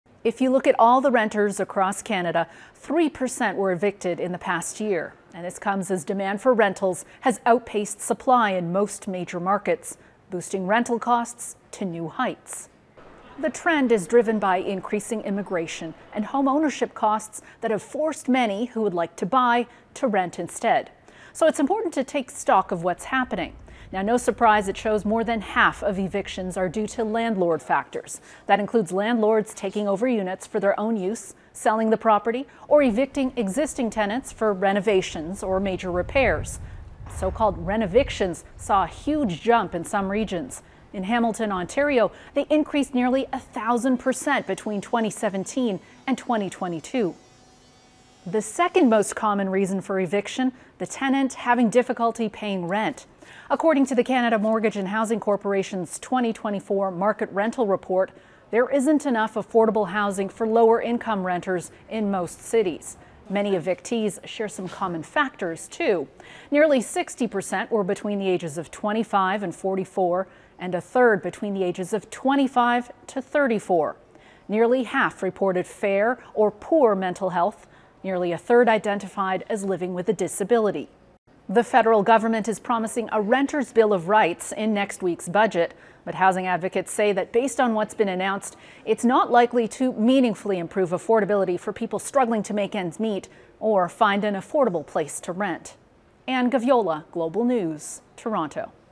Canada housing crisis: 3% of renters were evicted in past year
If you look at all the renters in Canada, three per cent were evicted in the past year and this comes as demand for rentals has outpaced supply in most major markets, boosting rental costs to new heights.

The trend is driven by increasing immigration and home ownership costs that forced many who were looking to buy to rent instead.

Anne Gaviola has more.